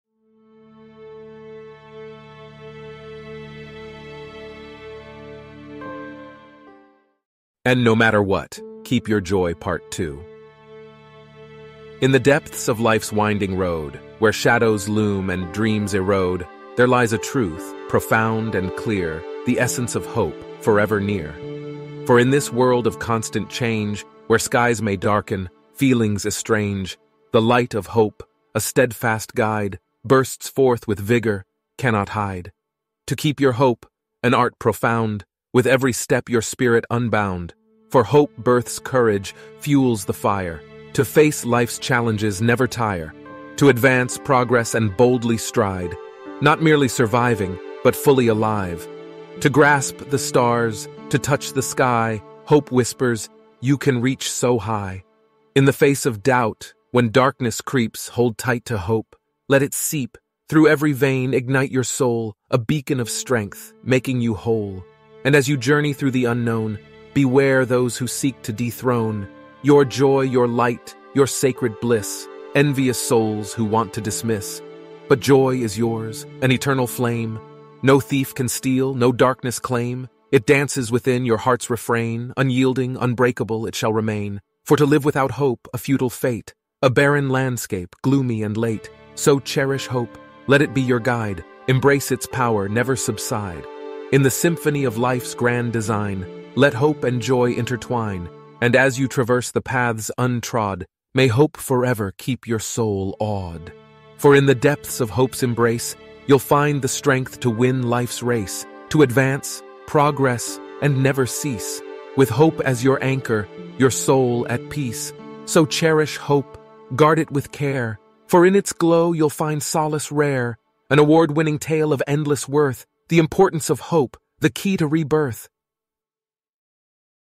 Keep Your Joy Pt. 2
One of the most important thing in the world that should be kept by a human is you JOY. For without it this life beyond hard, beyond complicated, and not enjoyable. Many will attempt to steal your JOY, but it is up to you whether or not they succeed or not. Situations will attempt also to steal our JOY, but we by no means and under no circumstances are we allowed to allow it to do so. The power of perseverance, endurance, and undeterred determination has been embodied deep within us by our Creator. It is entirely our duty and obligation to bring it out in times of need. To continue to strive we must, to regain our hope we must,  and most importantly to regain our JOY, indeed we must.